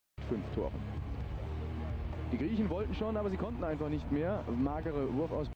Balonmano - Gol de espalda
Un gol con bastante riesgo (de fallo).
Hay que estar muy seguro para hacer algo así.